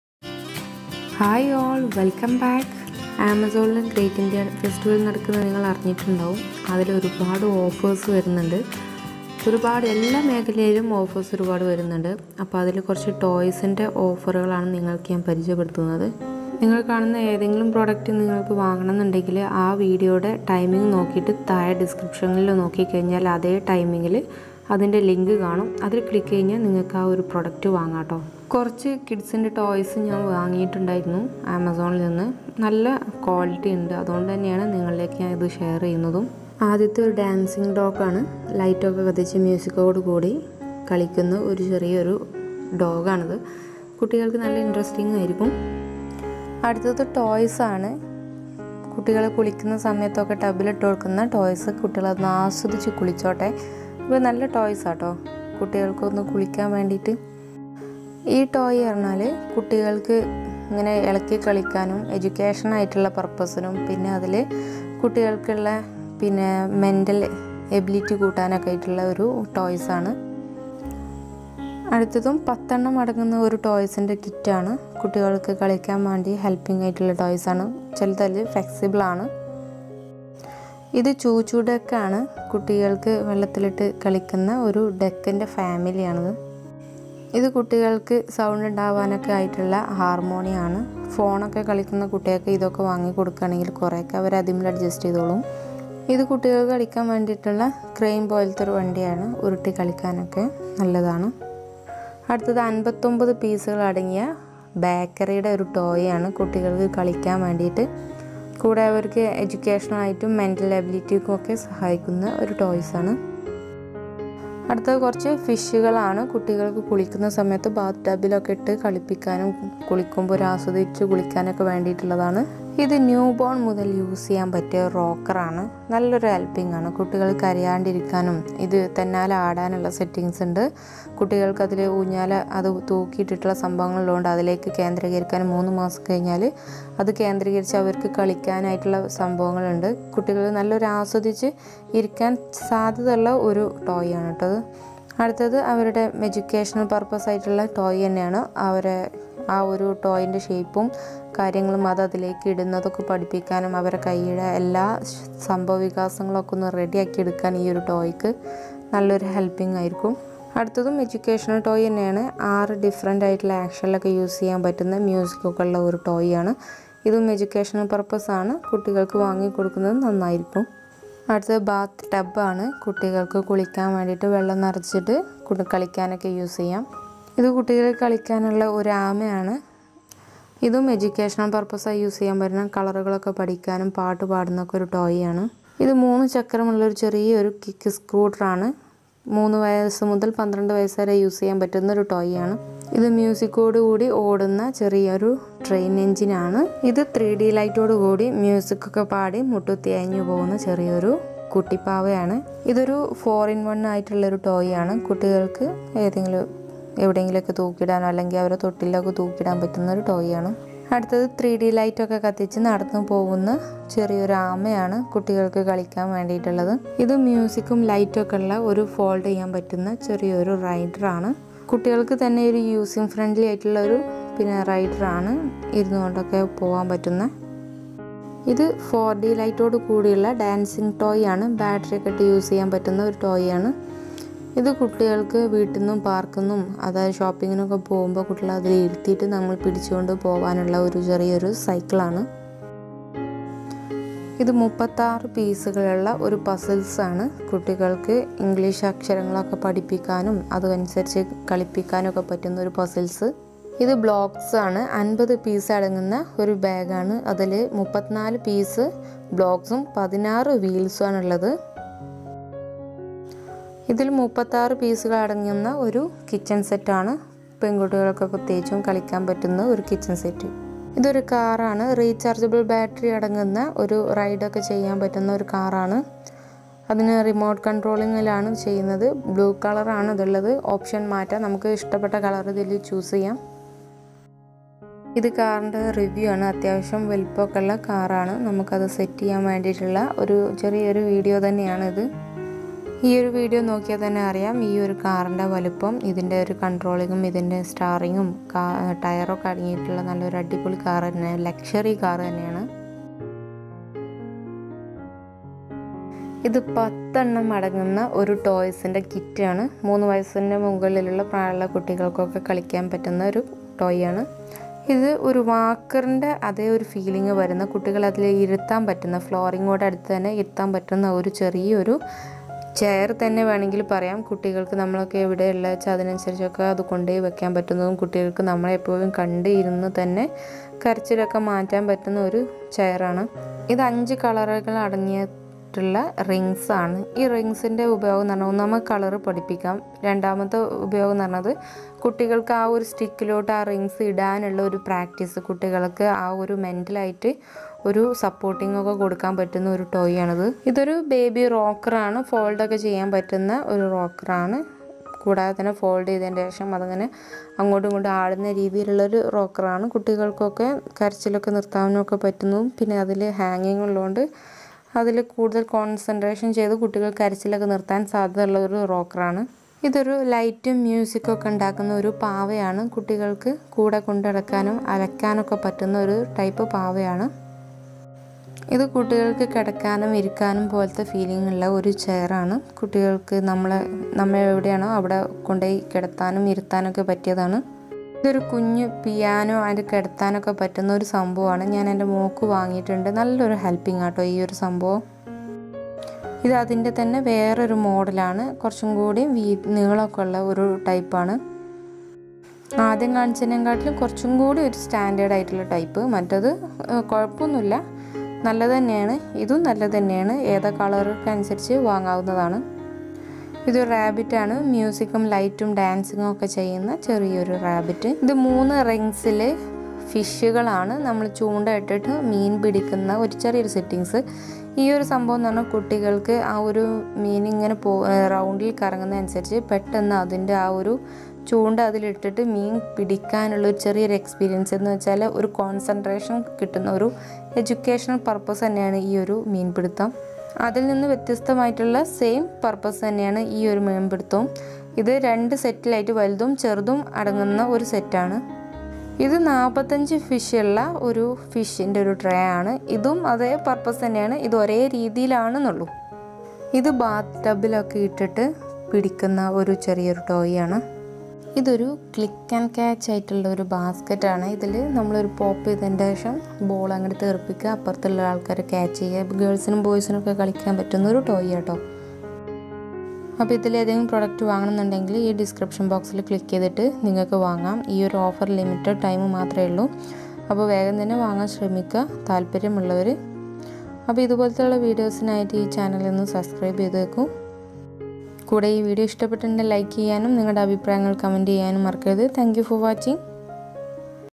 Amazon great india festival 2020| ഒരുപാട് baby toys ഇപ്പൊ വിലക്കുറവിൽ , ഈ അവസരം പിന്നെ കിട്ടില്ല
0:40. dancing dog

0:47 bath

1:10 baby toy

1:16  10 Peace toy

1:24 bath toy

1:39 piano kids

1:40 fire engine

1:45. sweet shop

1:58 bath toy fish

2:12 new born

2:27Shake and Grab Ball,
2:42 abcd

2:54 bath tab

2:58

3:03 4 in 1

3:07 Kids Skate Kick Scooter -

3:13Musical Bump and Go Smiley Train

3:18 Runing and Weeping Naughty Baby
3:24 4 in 1 thukkiyidunna toy

3:31 Turtle Toy with 3D Lights

3:37 Foldable Tricycle

3:50  Toy Vehicles

3:56 Children with Seat Belt Kid's Ride Outdoor

4:03 abcd sheet
4:15 block vandi

4:25 kitchen set

4:30 car

5:08 Vehicles Toy Cars

5:15 FunBlast Building Blocks for Kids with Wheel

5:34 colour rings

5:51  newborn to Toddler Musical Portable

6:10 cloth toy with Light

6:19 kids sitting

6:29 baby piano 3 month

6:41  baby piano 3 month 3 in 1
6:54 rabit

6:59 fishing game  Fish-Catching

7:21 fishing game Fish-Catching

7:31 fishing game  Fish-Catching

7:38 bath toy

7:42 click  and catch

Instagram .nishanaadukkath